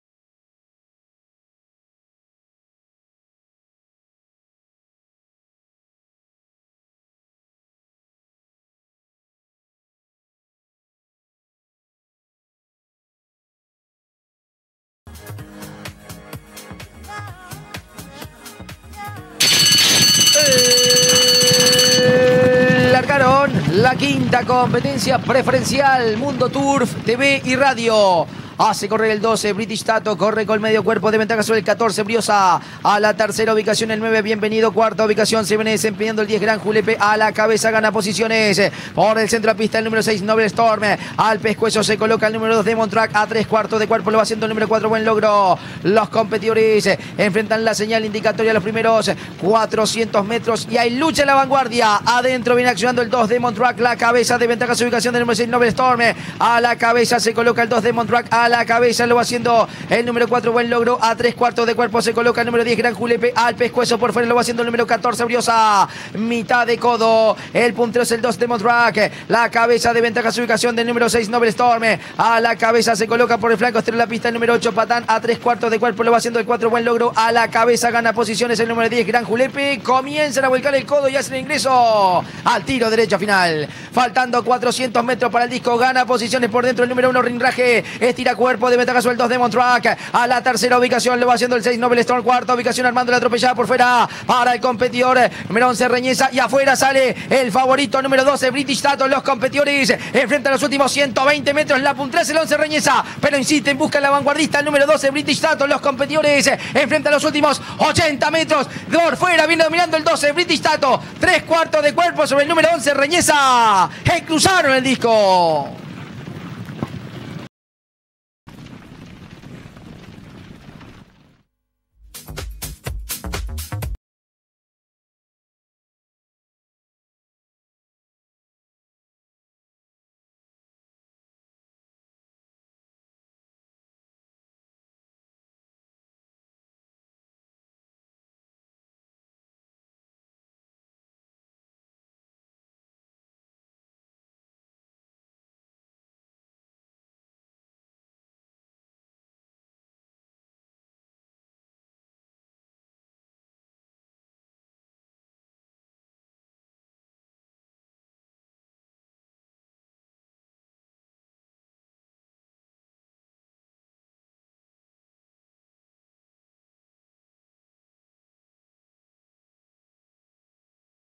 18/07/2017 – Hipodromo Paysandu – Carr 5 – MUNDO TURF (TV Y RADIO)
Carrera 5 realizada el dia 18/07/2017 por el premio MUNDO TURF (TV Y RADIO) en el Hipodromo Paysandu